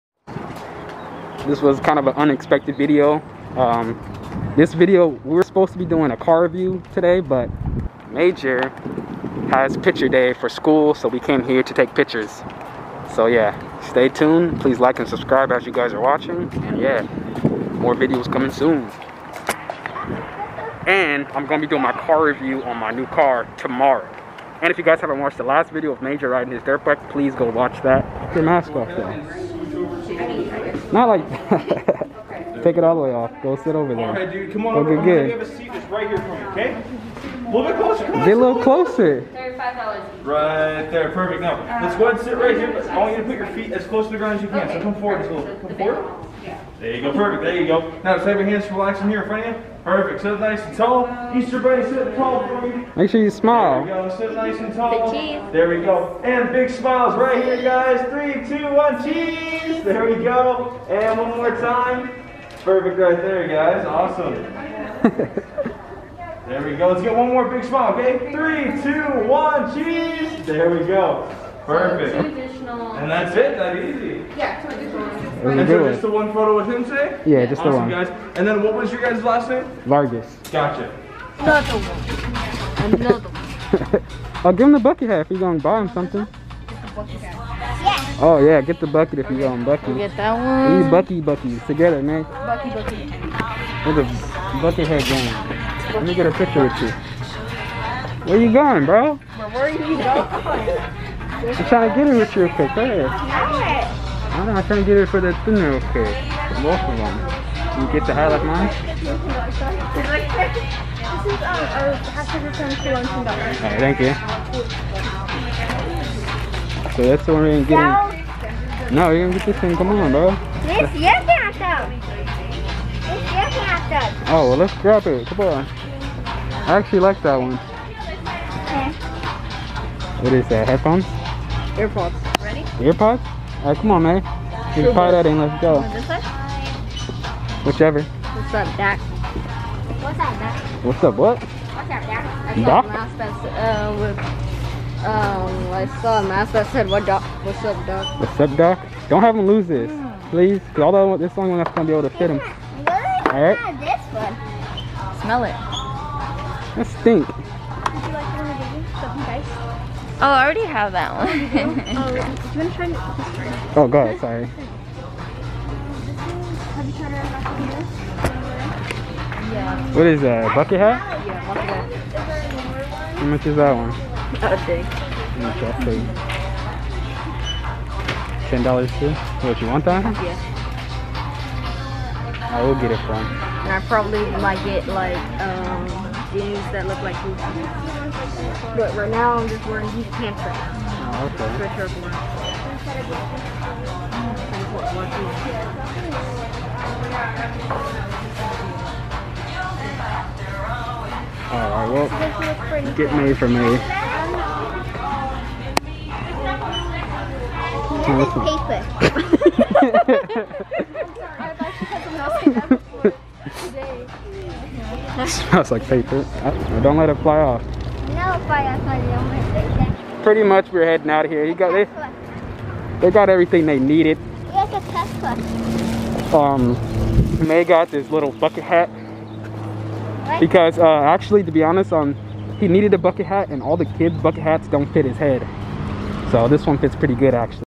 Picture Day(Life With The Vargas Family)
Video Published April 7th 2021

picture day for my son